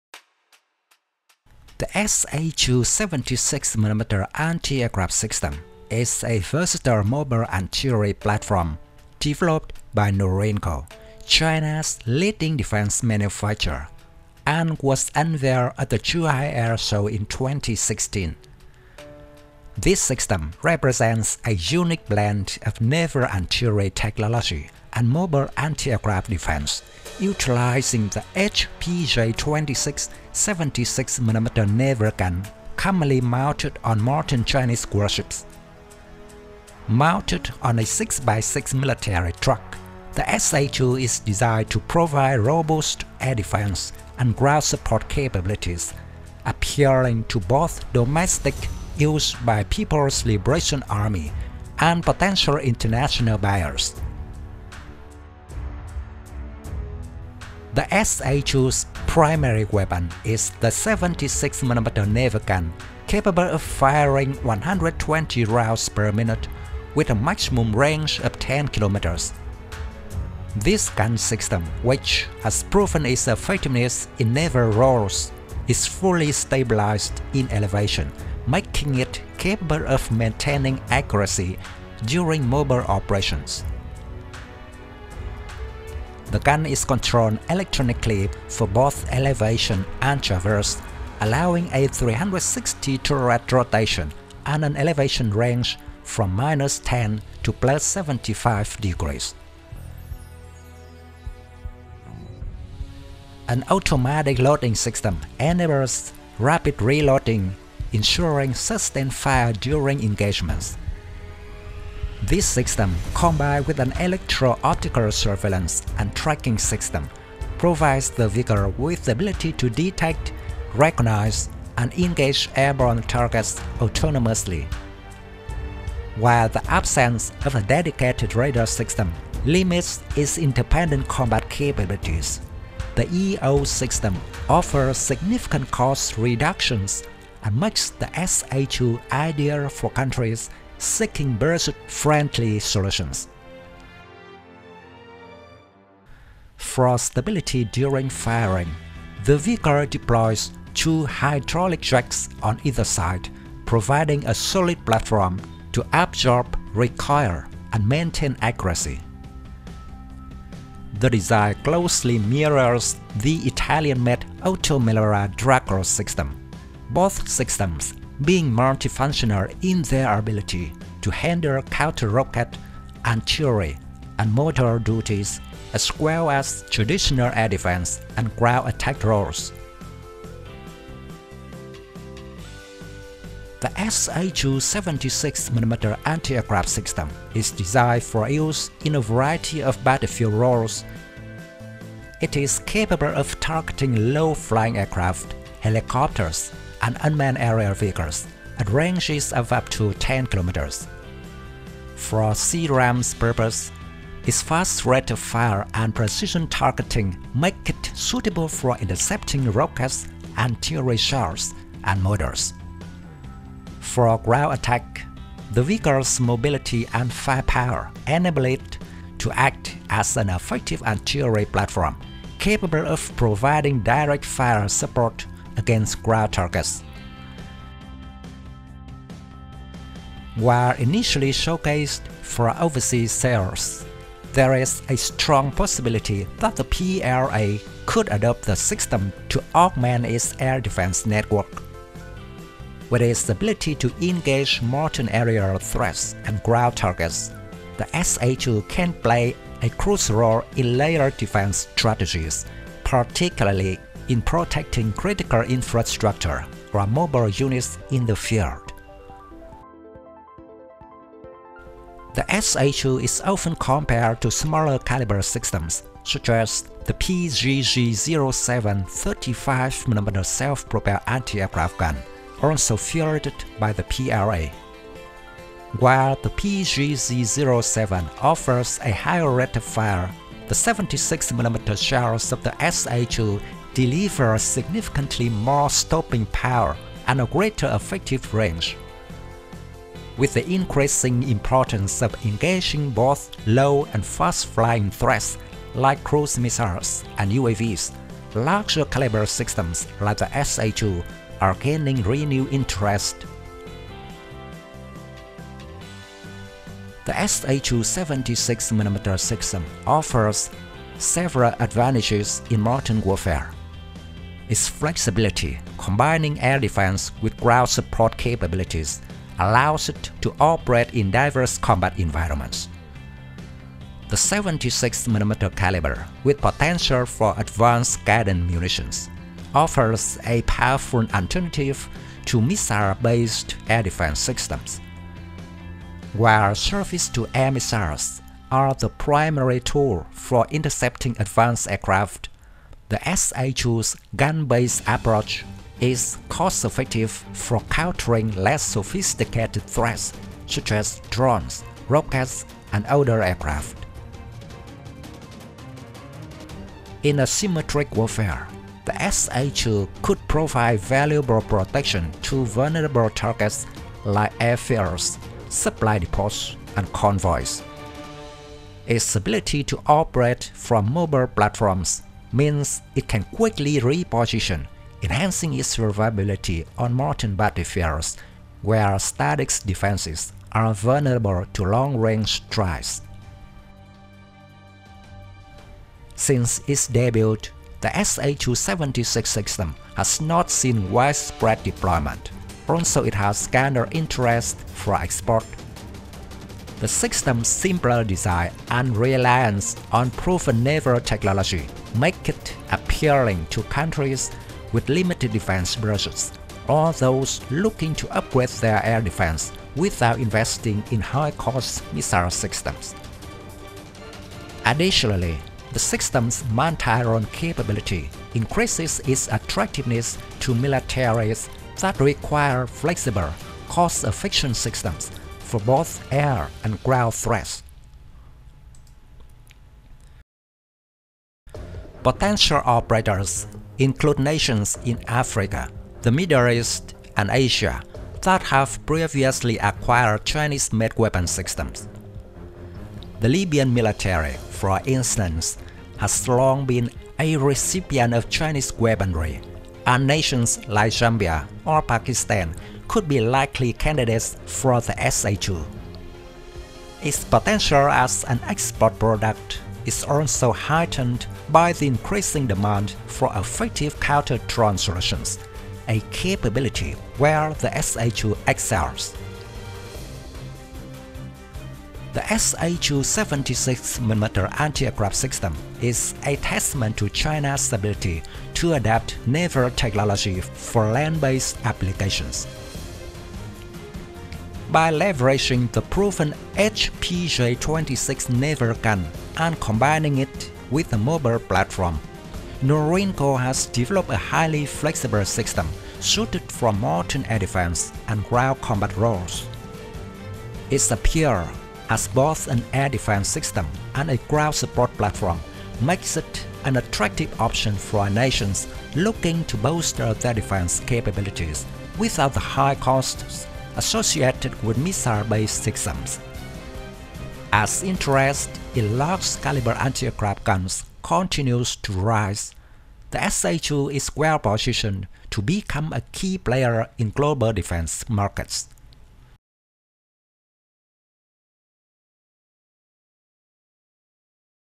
SA2 76mm Anti-Aircraft System: China's Versatile Defense Powerhouse
Discover the SA2 76mm Anti-Aircraft System, a versatile mobile artillery platform developed by China's defense giant, NORINCO. Unveiled at the 2016 Zhuhai AirShow, this system combines naval artillery technology with mobile air defense, featuring the powerful H/PJ-26 76mm naval gun commonly seen on Chinese warships. Mounted on a 6x6 military truck, the SA2 offers robust air defense and ground support, capable of firing 120 rounds per minute at targets up to 10 kilometers away. With advanced stabilization, rapid reloading, and an Electro-Optical (EO) tracking system, the SA2 is a budget-friendly yet effective solution for both domestic and international defense needs. Learn how this system plays a crucial role in air defense, C-RAM duties, and ground attacks while providing cost-effective protection against modern aerial and ground threats.

Thank you very much.
Best regards
Dung Tran.